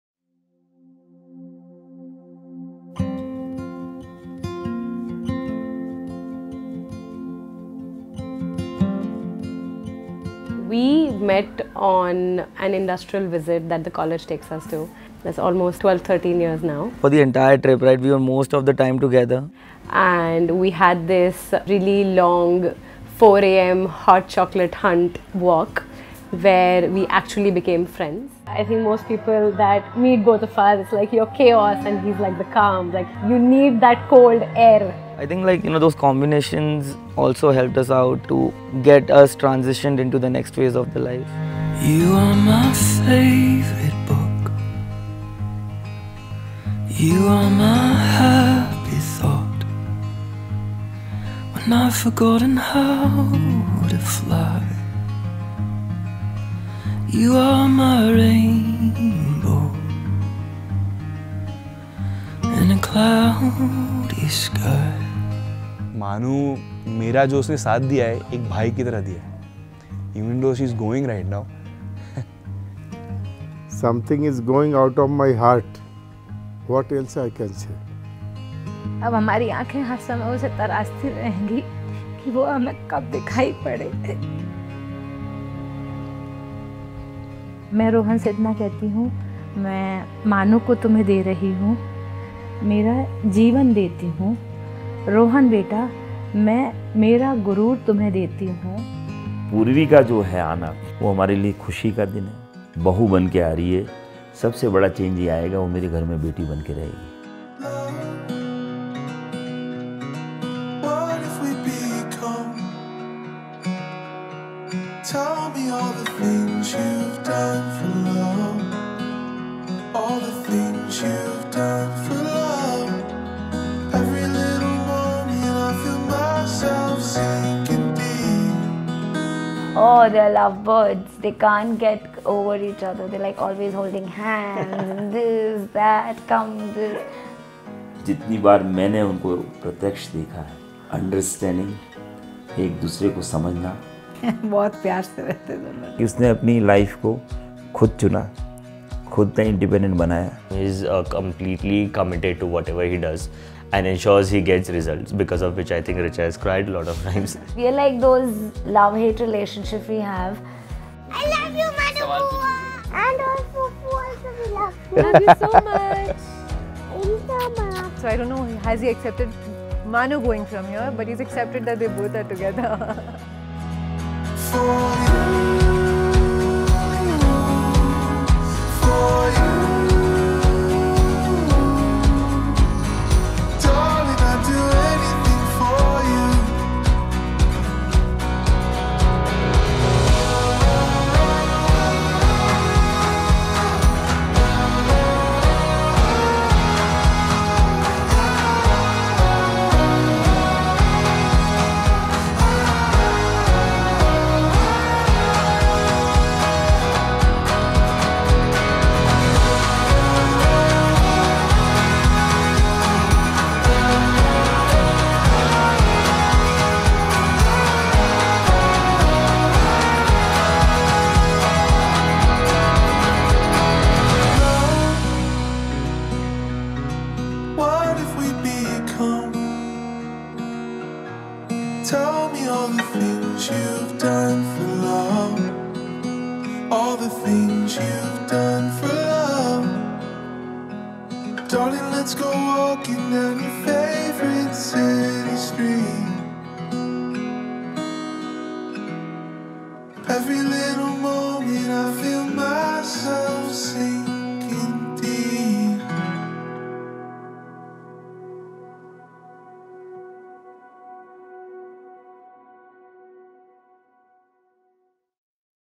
Little joys in life | Purvi & Rohan | Le meridien, Mahabaleshwar
Purvi and Rohan’s wedding was the perfect example of ‘Love is Friendship’. Their story started off as friends and ended up with them planning to spend the rest of their lives with each other. The celebrations started in the ever-lovely Marriott with a fun engagement ceremony and was elevated with all the other functions taking place in the serene Le Meridian, Mahabaleshwar with perfect weather! It was a week full of fun, laughter, happiness, and of course, love. Their first look was the most special, complete with tears and mutual adoration for each other. Watch their trailer to know more about this cute couple…in HD & full volume!

Film by  @WeddingNama
Creative Head: @AnkitaAsthana
Production Head: @akash-agarwal
WN Bride: Purvi
WN Groom: Rohan
Planners: @celebrationeventswll
MUA: @dollyouup_bys
Location : Le meridien, Mahabaleshwar

Shot on Nikon Z6ii & Nikon Z9 mirrorless cameras

A WeddingNama production WeddingNama​